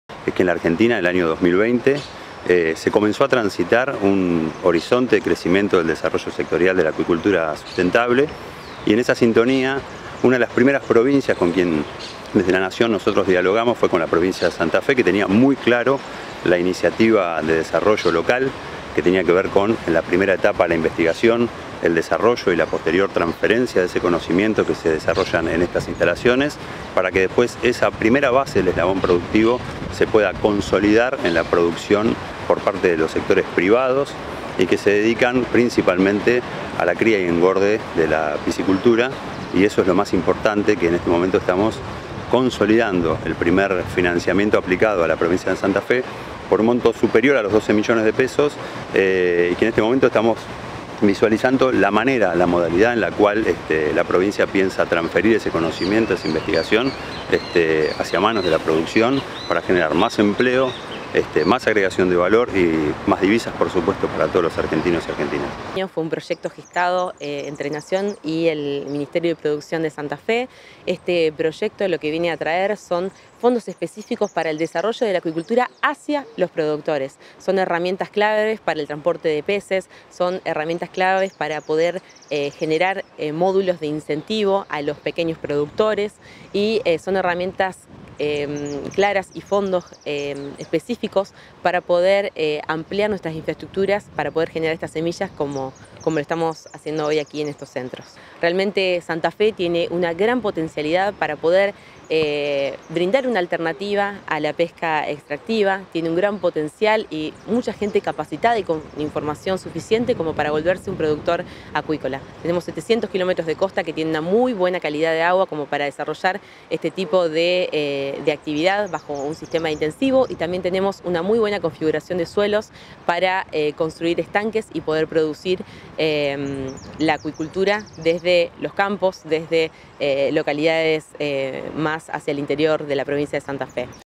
SANTA FE RECIBIRÁ CASI $13 MILLONES DEL FONDO NACIONAL DE ACUICULTURA
GUILLERMO ABDALA – DIR. NACIONAL DE ACUICULTURA

ELIANA EBERLE – PROG. ACUÍCOLA PROVINCIAL